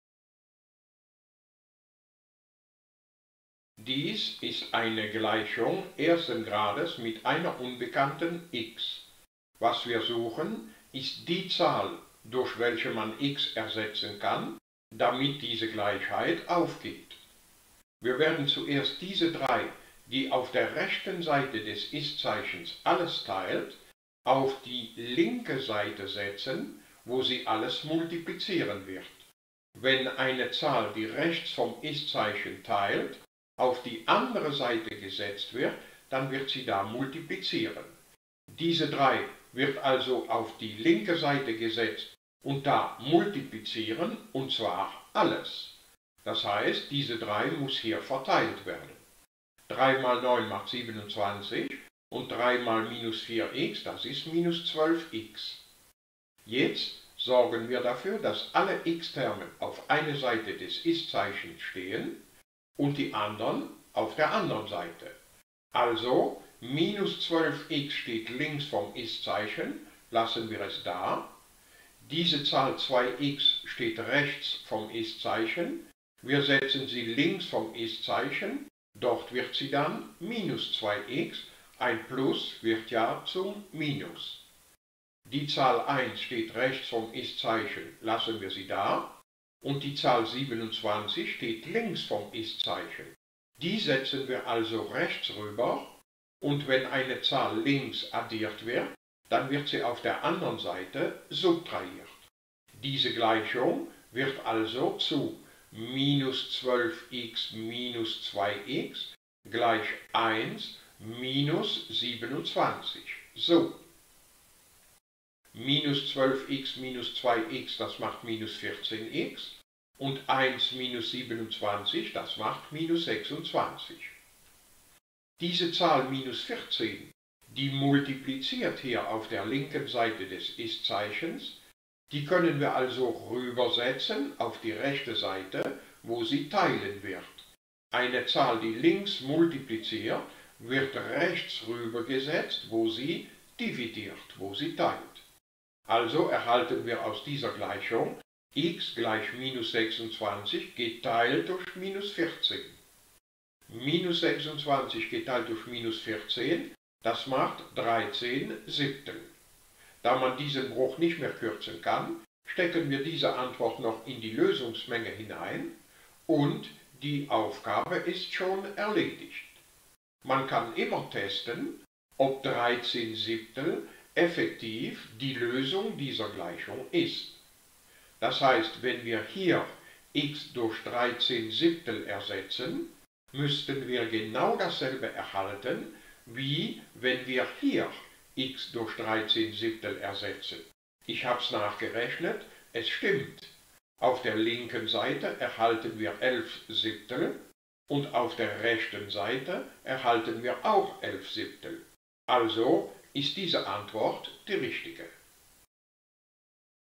Gleichungen ersten Grades 002
Wir lösen hier eine Gleichung ersten Grades mit einer Unbekannten und überprüfen die Antwort.